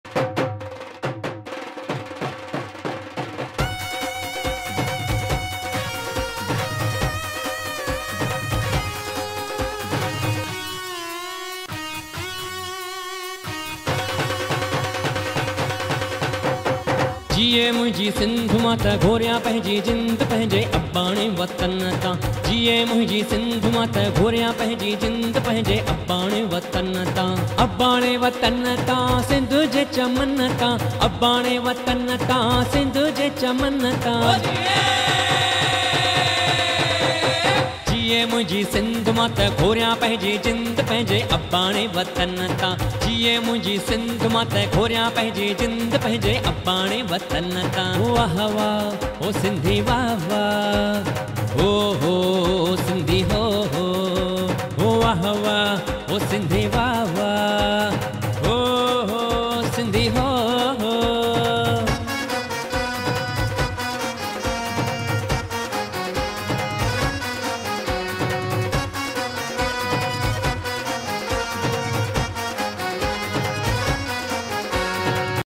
Cheti Chand/ Jhulelal Jayanti Celebrations 2022 - Part 1
Cheti Chand (चेटीचंड,  چيٽي چنڊ‎) Celebrations at Convention Centre, Indira Nagar, Lucknow